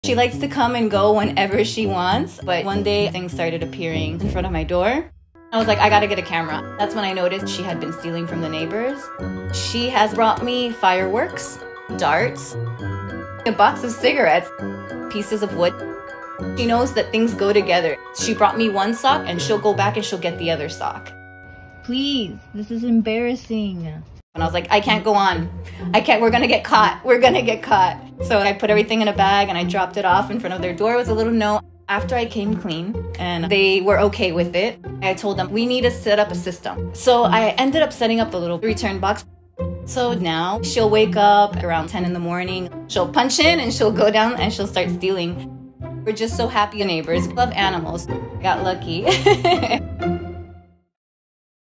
Cat Keeps Stealing From The Neighbors l The Dodo
This cat stole stuff from the neighbors to gift to her mom 🤣